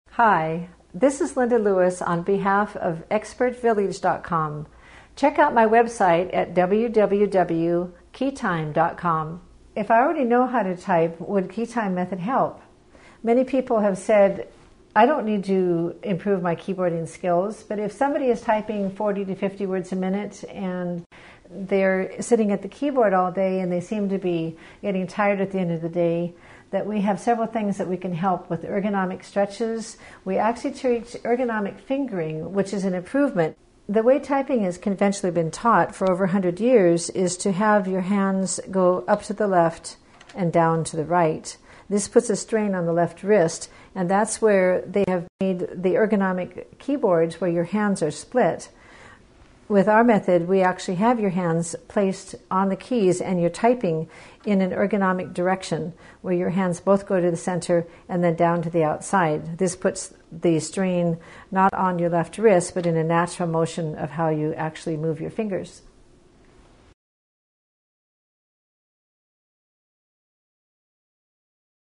Ergonomic Hand Placement for Typing
Ergonomic Hand Placement for Typing. Part of the series: Computer Typing Lessons. Where do I put my hands to type? Learn about ergonomic hand placement in keyboarding  in this free video.